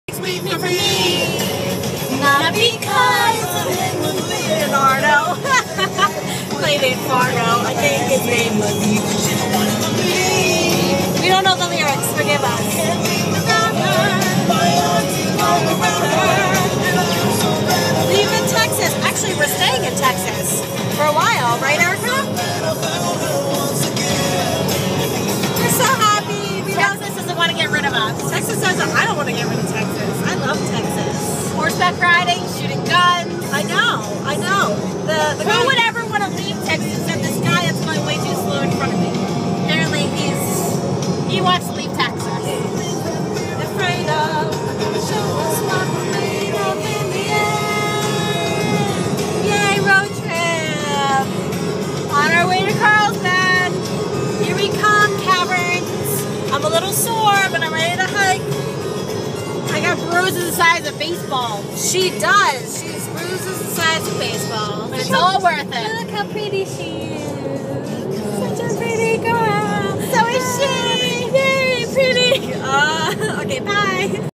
On the road again
We drive to the caverns bye bye Texas